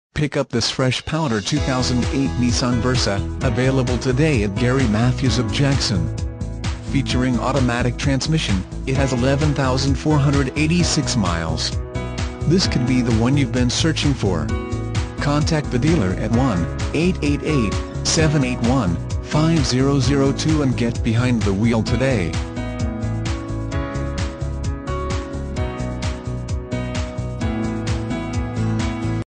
2008 Nissan Versa
Pick up this Fresh Powder 2008 Nissan Versa, available today at Gary Mathews of Jackson. Featuring Automatic transmission, it has 11486 miles. This could be the one you've been searching for. Contact the dealer at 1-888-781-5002 and get behind the wheel today!